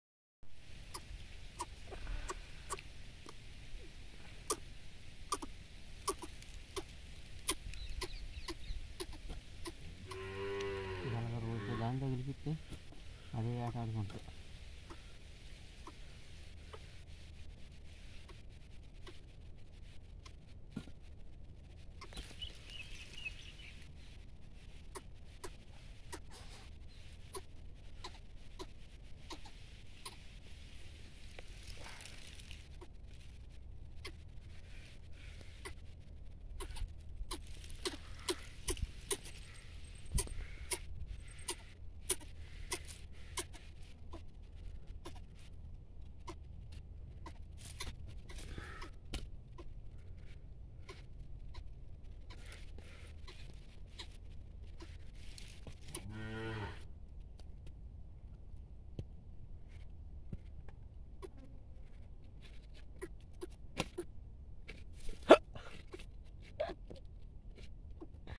hunting and eating itself greater coucal bird @bharath kumar 1998
in this video I have shown the greater coucal baby eating itself